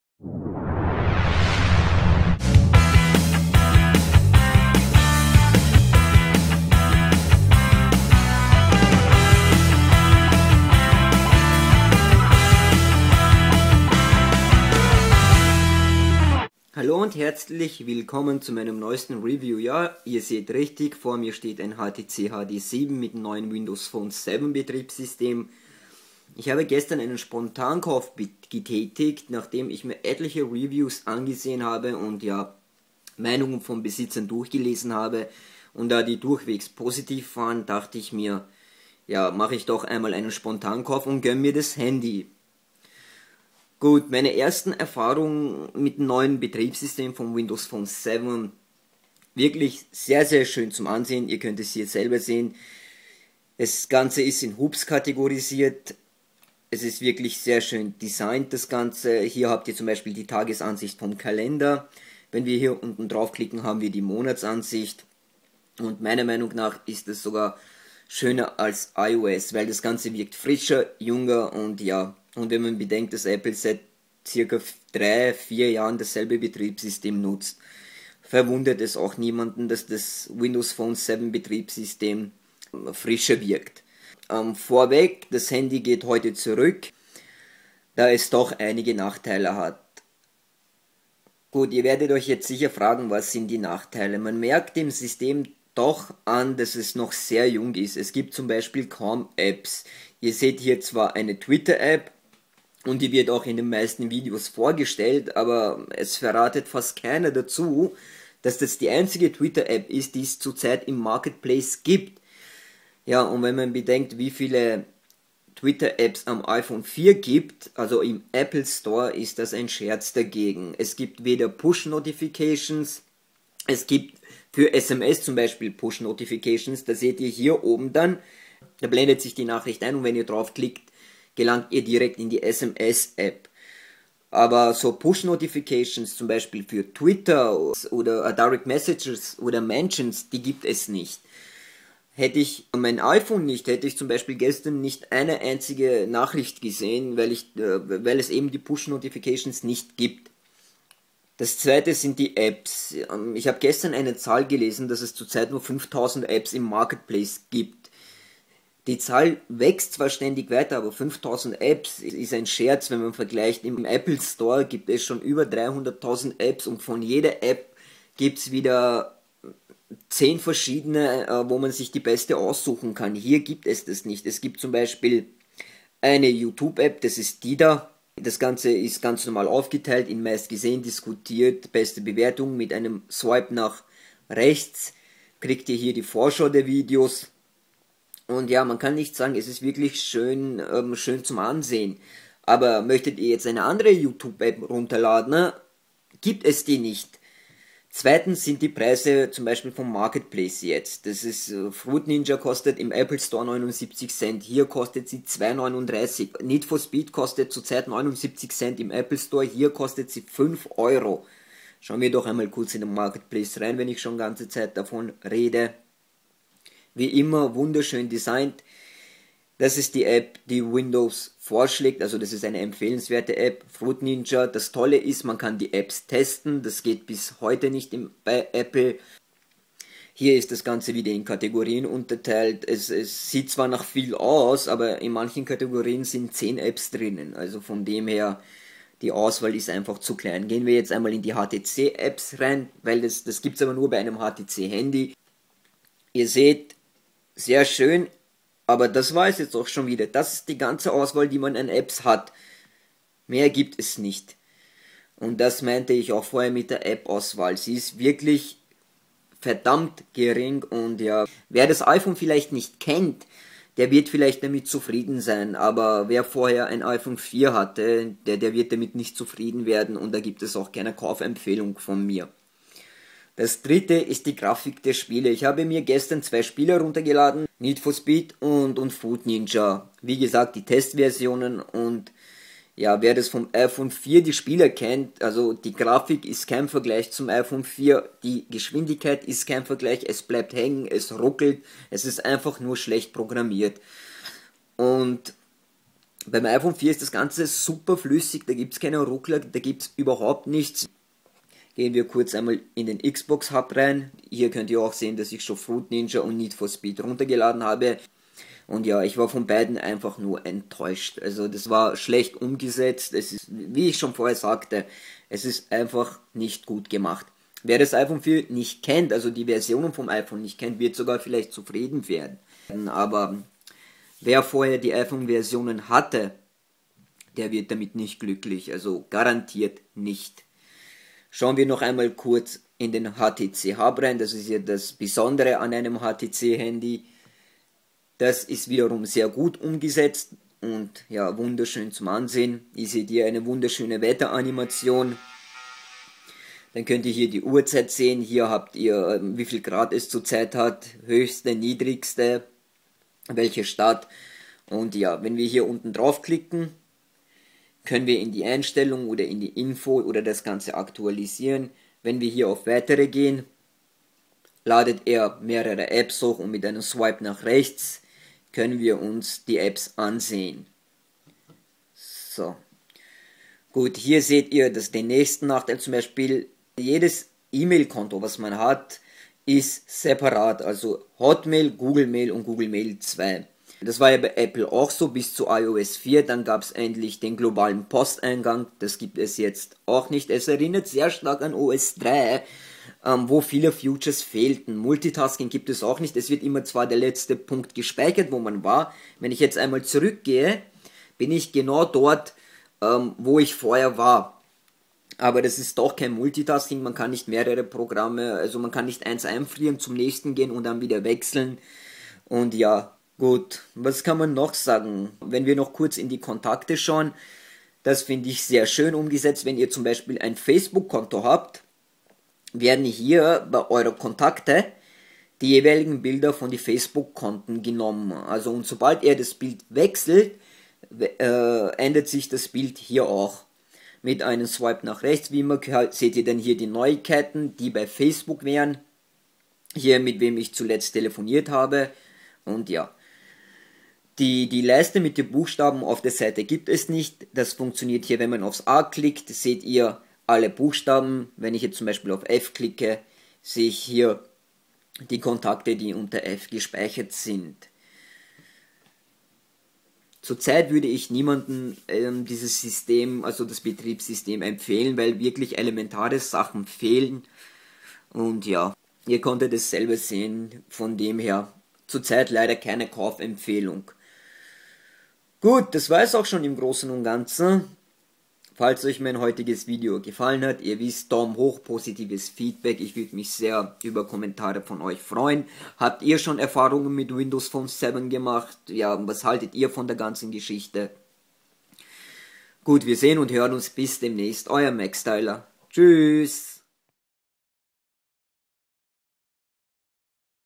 HTC HD7!
Hallo  heute stelle ich euch das Handy HTC HD7 mit dem brandneuen Windows Betriebssystem WP7 vor.
In den meisten Videos werden nur die positiven Seiten von WP7 gezeigt, ich gehe in meinem heutigen Video auch auf die weniger positiven Sachen ein so das man sich wirklich ein gutes und neutrales Bild von dem neuen Betriebssystem machen kann,so jetzt aber genug geschrieben ich wünsche euch viel Spaß beim zusehen.